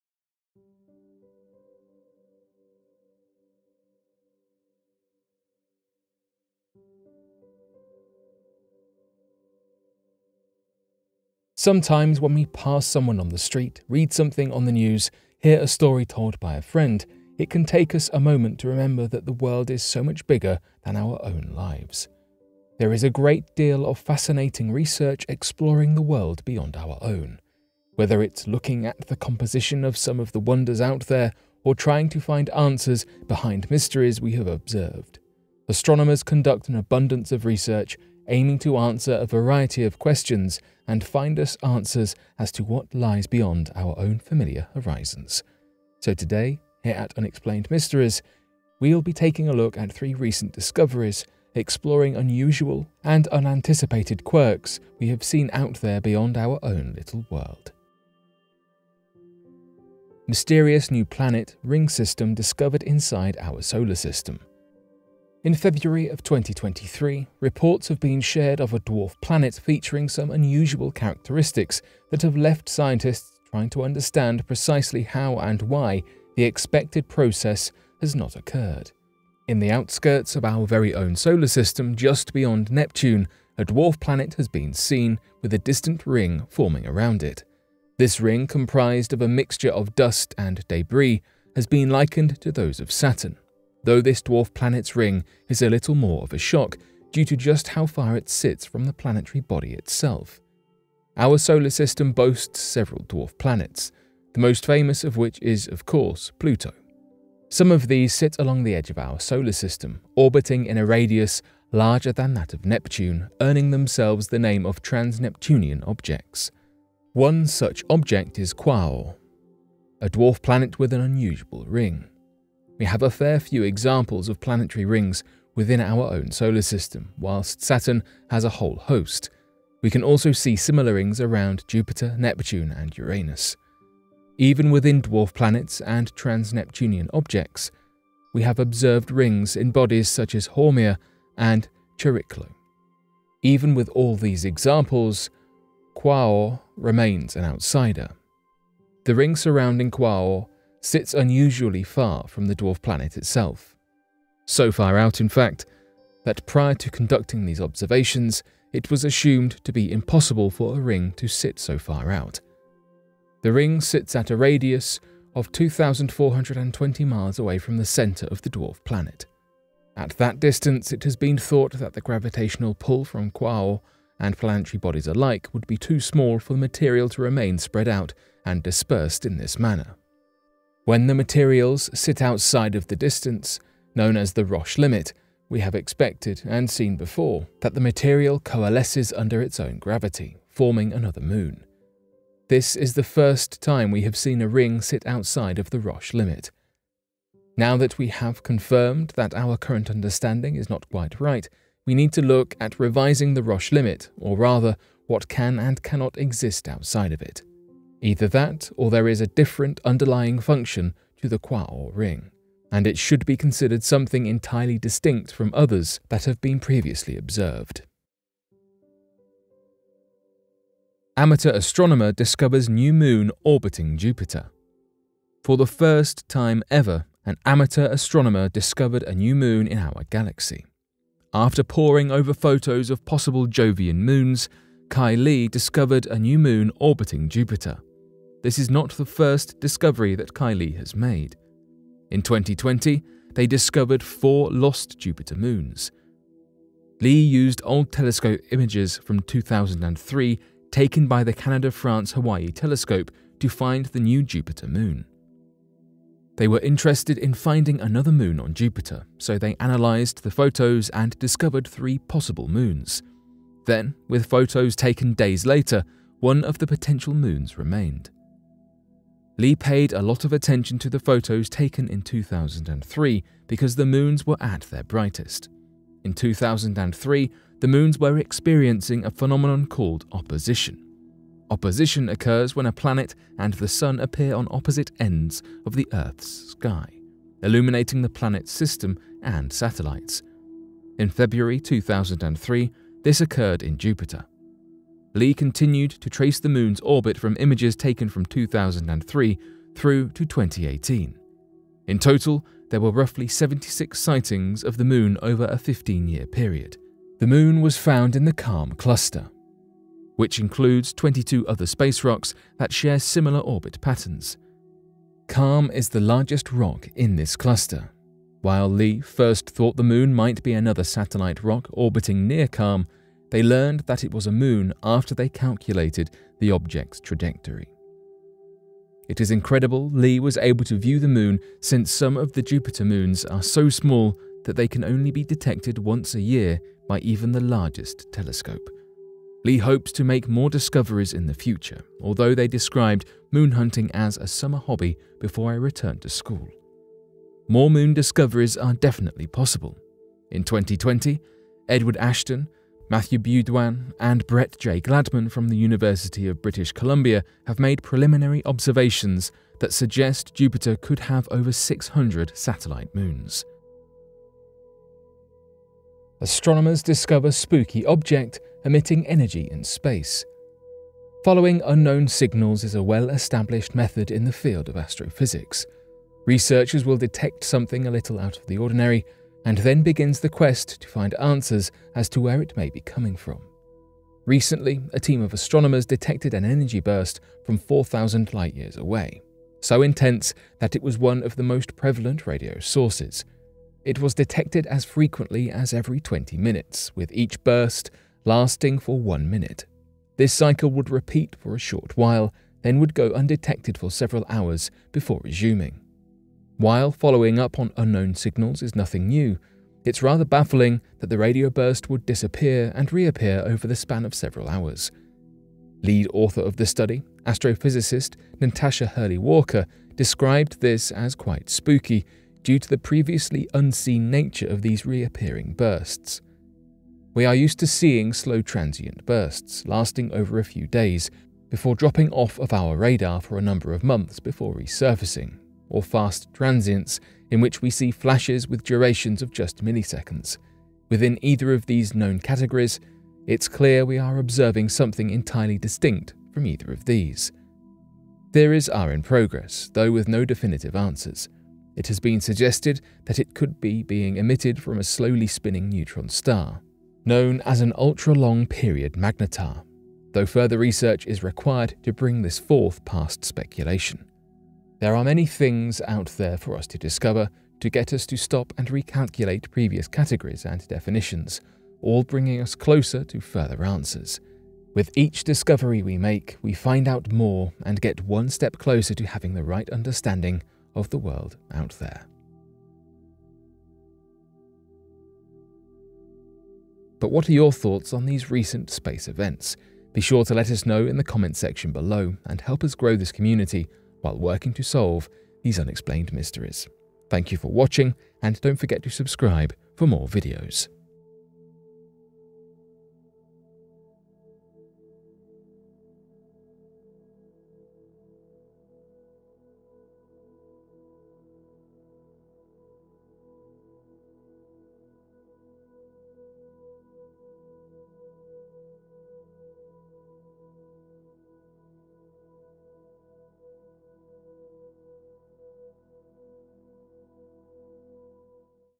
Scientists Just Revealed A Massive Ringed Object Has Been Observed Entering Our Solar System
Scientists just revealed a massive ringed object has been observed entering our solar system. Today, we take a look at this massive ringed object that entered our solar system.

For a lot of the research that we conducted, we make observations, look for patterns and trends, and then draw conclusions based off this. This scientific method has let us predict outcomes and formulate rules, theories, and understandings that follow. Eventually, these rules become understood as the explanation, a predictable manner allowing us to make a bit more sense of a confusing rule.